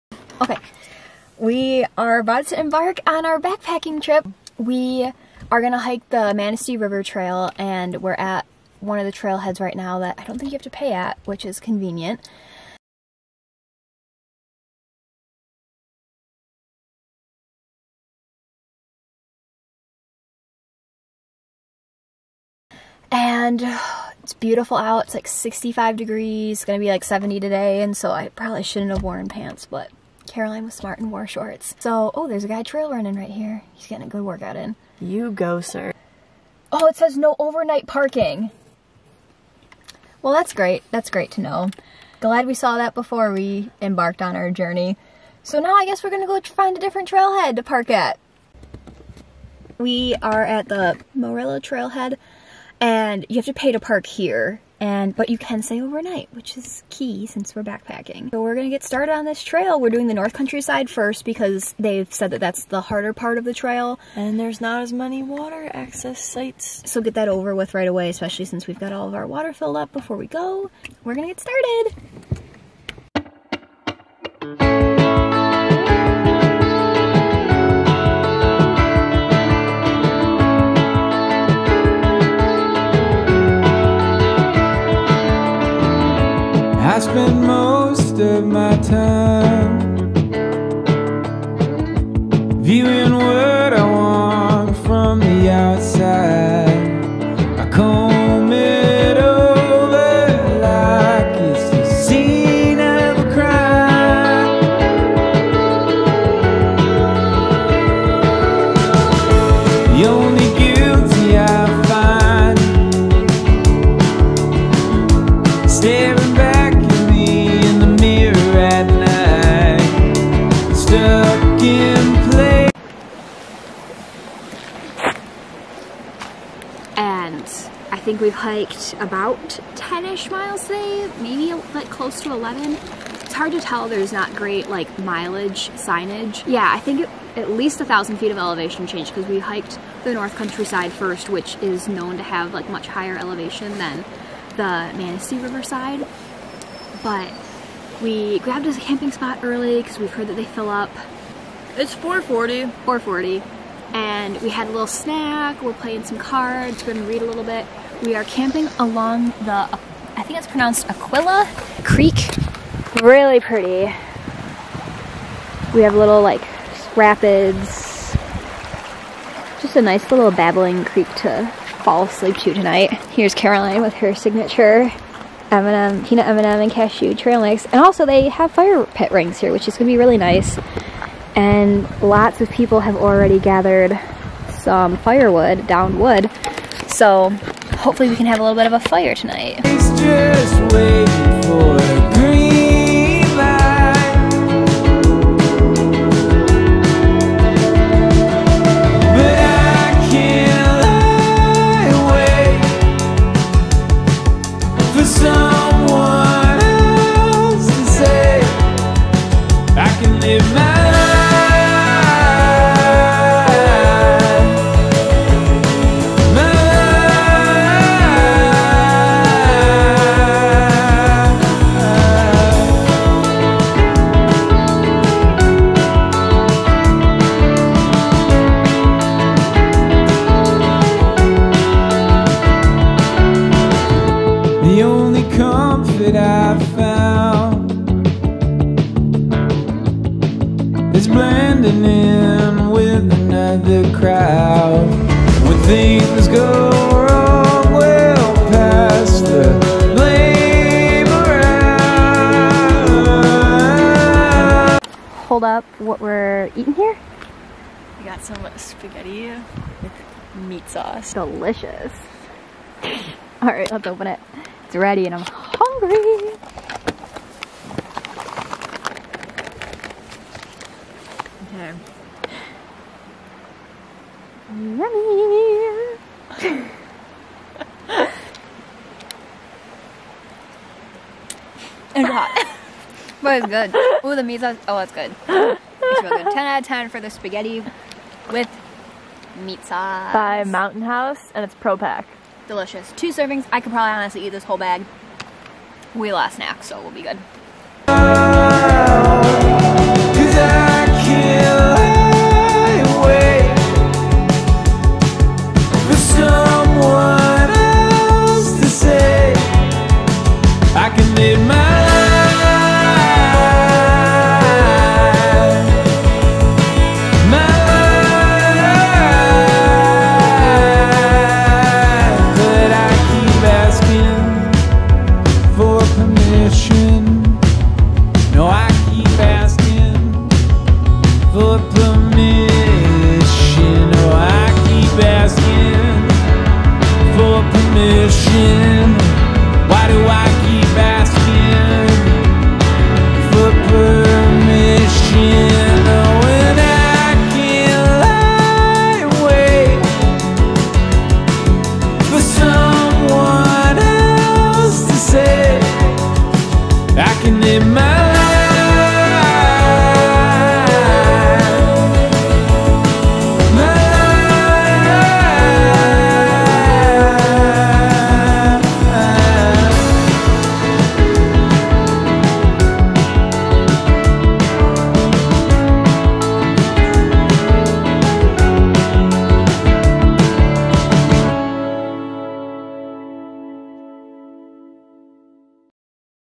BACKPACKING THE MANISTEE RIVER TRAIL | MICHIGAN BACKPACKING
22 miles of backpacking condensed into 6 minutes!

Come along with me and my sister while we backpack one of the most popular backpacking loops in Michigan. The Manistee River Trail is a gorgeous hike along the Manistee River. With a number of designated campsites and dispersed camping allowed, this ~20 mile loop is the perfect weekend trip!

Tips and Tricks:
- Hike the NCT side of the trail first (more elevation change and only 1 water source available on that side)
- Go on a weekday to avoid crowds
- If hiking on the weekend, aim to set up camp early to guarantee a good spot